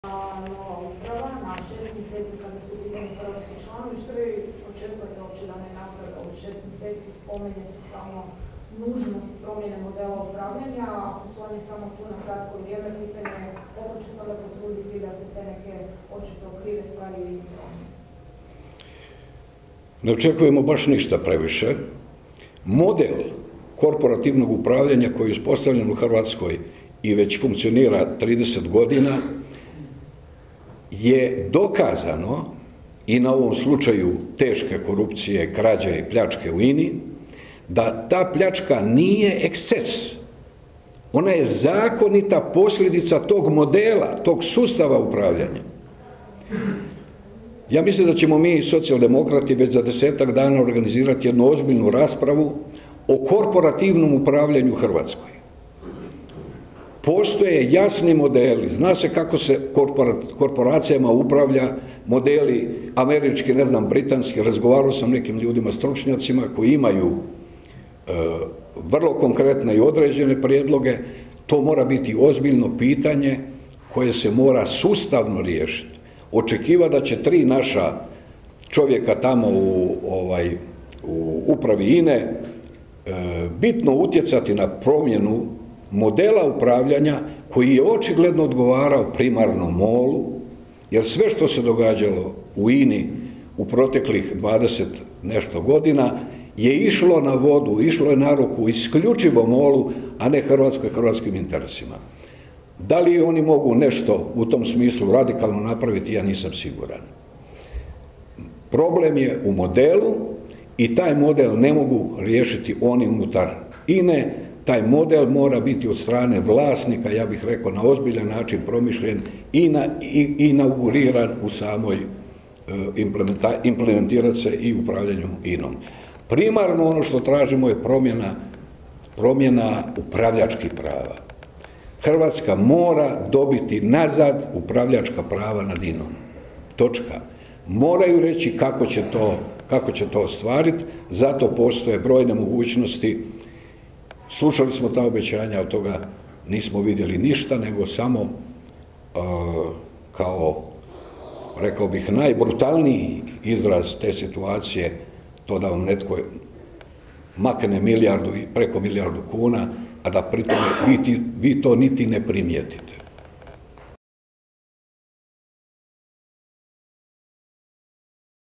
Vidović: "Sve što se događa u Ini zadnjih 20 godina ide na ruku MOL-u"
Na konferenciji za medije kluba zastupnika Socijaldemokrata održanoj u Saboru o temi "Na što predsjednik Vlade nema odgovor?" govorili su zastupnici Davorko Vidović, Domagoj Hajduković i Ivana Posavec Krivec.